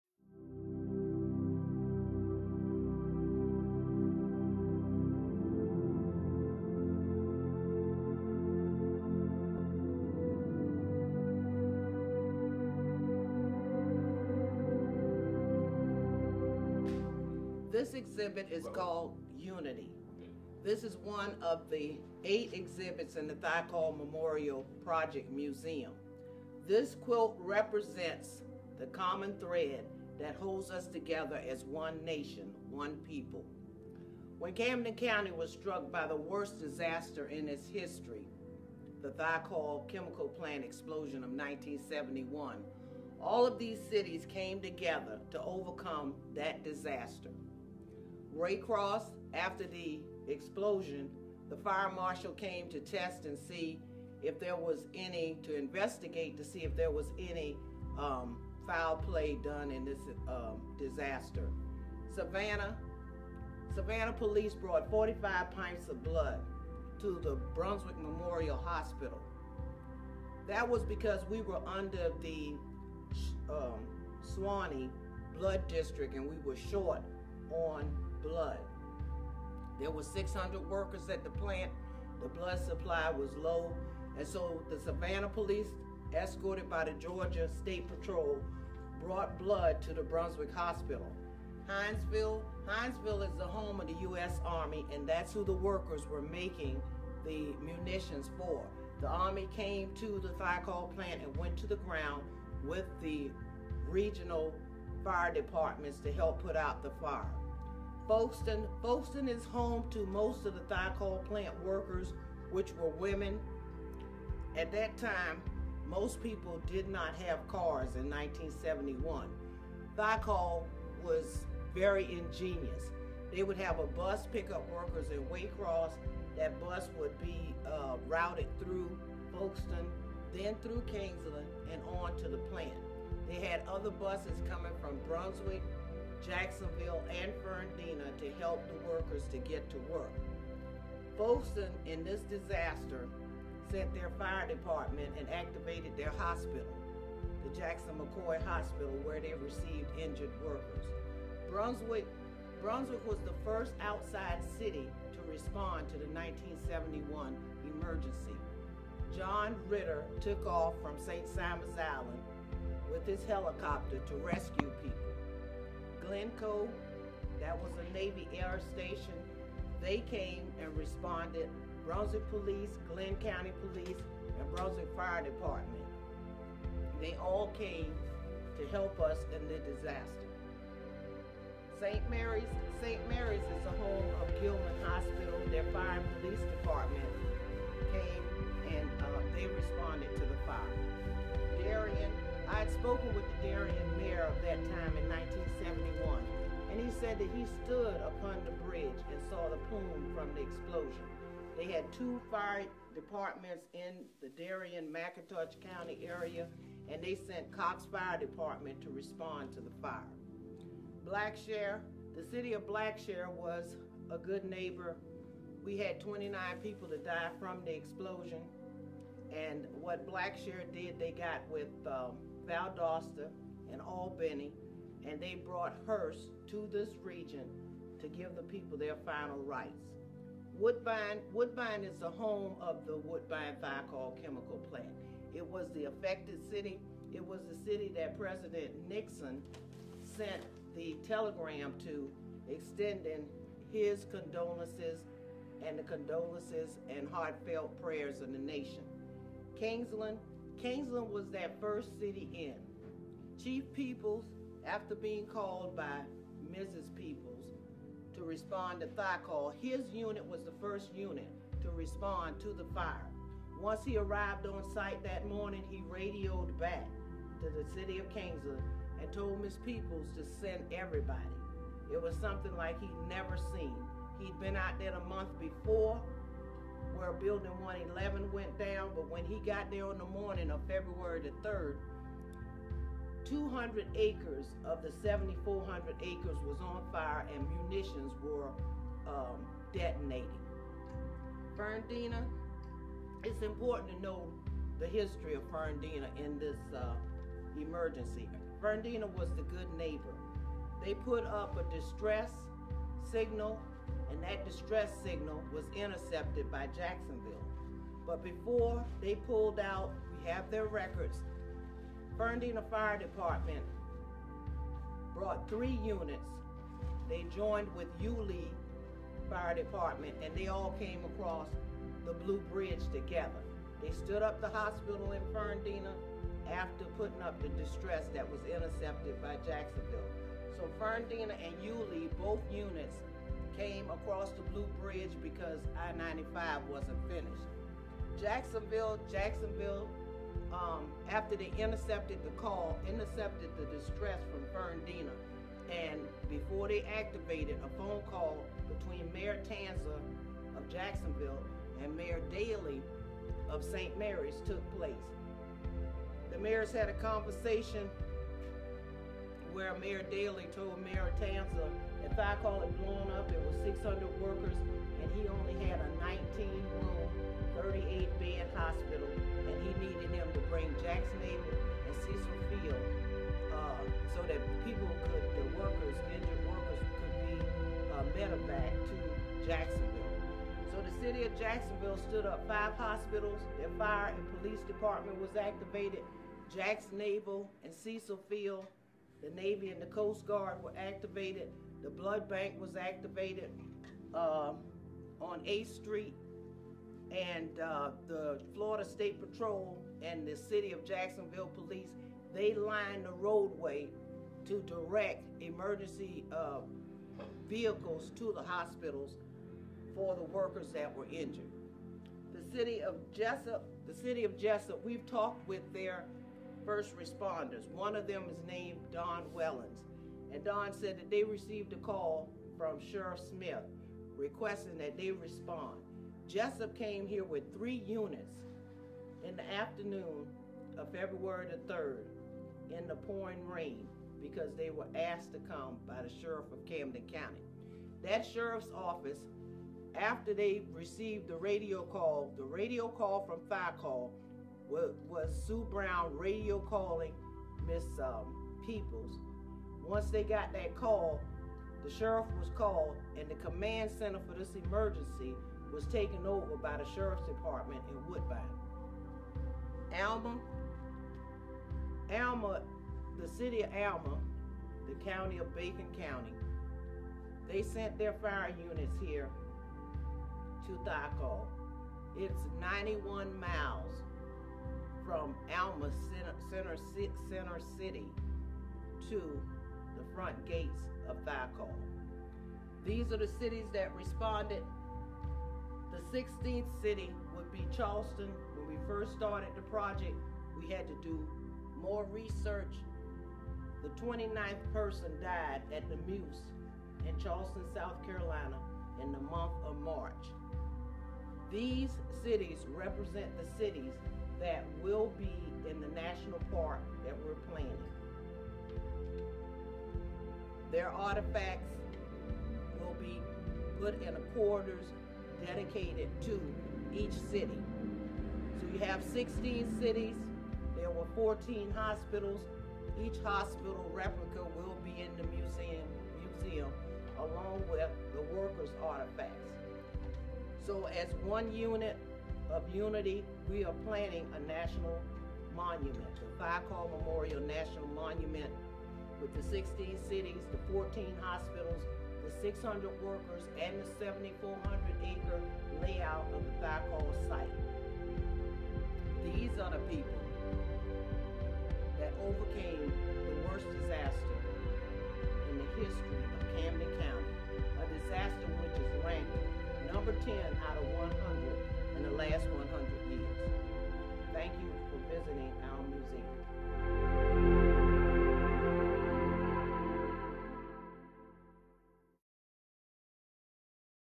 Come in Unity
In this video you will learn about the 16 city who came together in unity to save all the lives possible.

Thiokol Memorial Project introduces a virtual tour of The Thiokol Memorial Museum. The Museum serves the foremost depository of the history of Woodbine Thiokol Chemical Plant from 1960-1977. The history which paved the way to modern America.